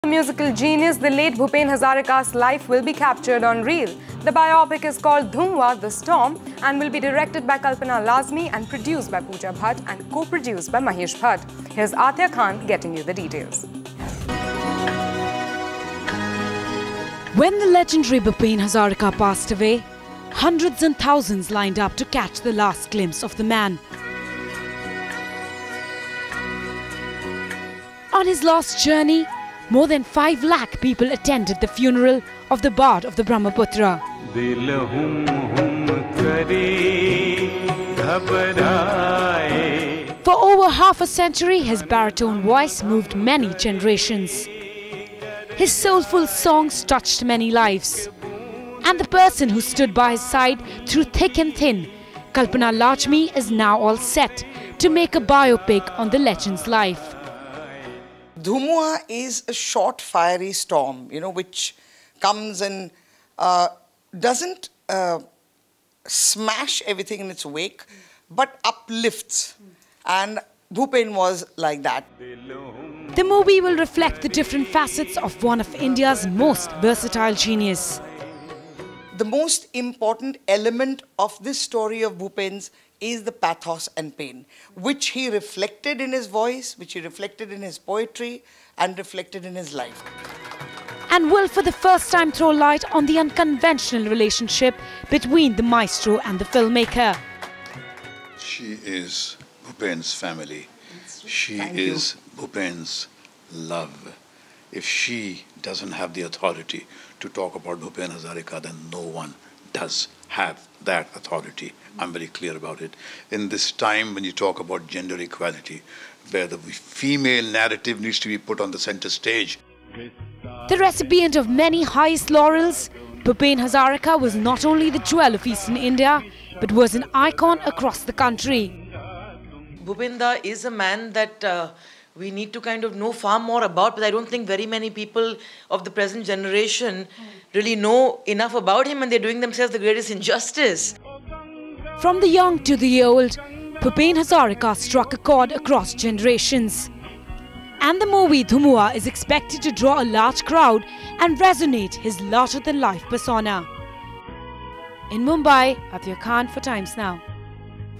Bhupen Hazarika's life on celluloid
The late legendary musician Bhupen Hazarika's life is all set to be captured on reel. The biopic is called 'Dhumua: The Storm' and will be directed by Kalpana Lajmi. The film will be produced by Pooja Bhatt and co-produced by Mahesh Bhatt. The biopic will be based on the passionate 40-year-old relationship between Bhupen Hazarika and Kalpana Lajmi. Download the Times Now India's Election HQ app and get all the election info at one go. Social Media Links